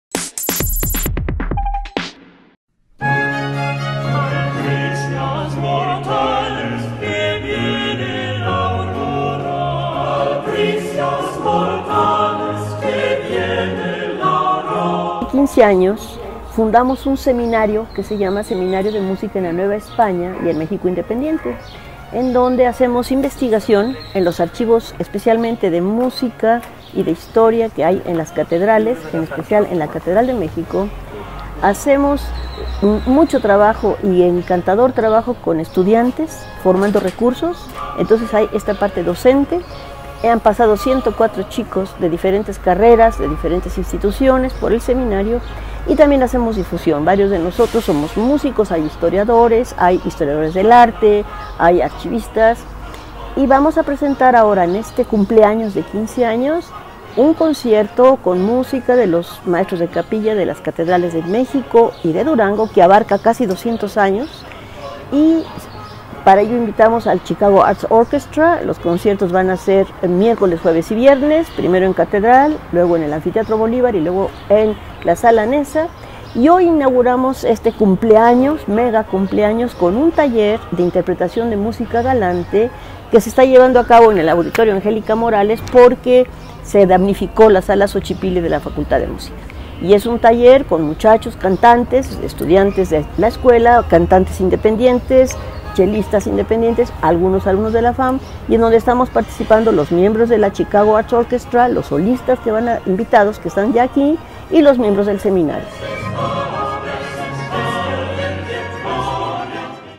UNAM celebrará décimo aniversario de CU como Patrimonio Mundial
A través de varias actividades culturales se celebrará X aniversario de la inscripción en la lista del patrimonio mundial del Campus Central de la Ciudad Universitaria. Entre ellas, destaca un concierto conmemorativo para festejar el XV Aniversario del Seminario de Música en la Nueva España y el México Independiente y del Sistema Musicat.